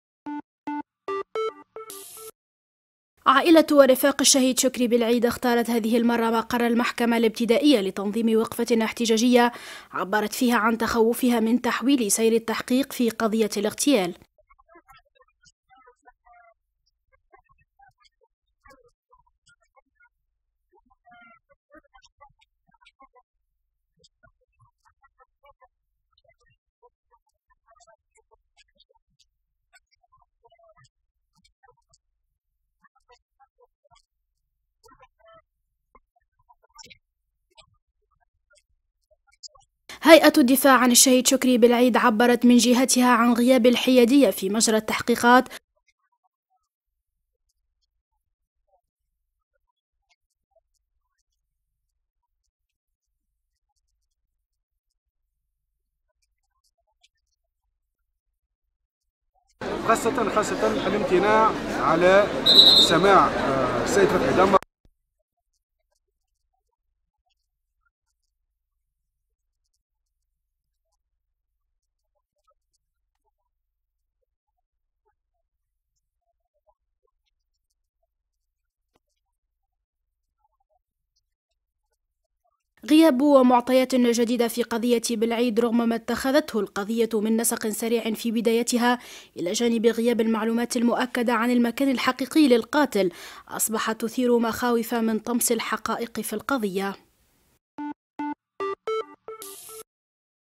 وقفة أحتجاجية لعائلة الشهيد بلعيد و أنصار الجبهة
نظمت عائلة الشهيد شكري بلعيد و انصار الجبهة الشعبية وقفة احتجاجية امامم المحكمة الابتدائية بتونس للمطالبة بالكشف عن قتلة الشهيد و لمطالبة قاضي التحقيقق بالتخلص من الضغوطات المسلطة عليه و تحمل مسؤوليته كاملة في الكشف عن قتلة الشهيد

تقرير فتحية الحرابي
تصوير :سهير البهلول
مونتاج محمد المنستيري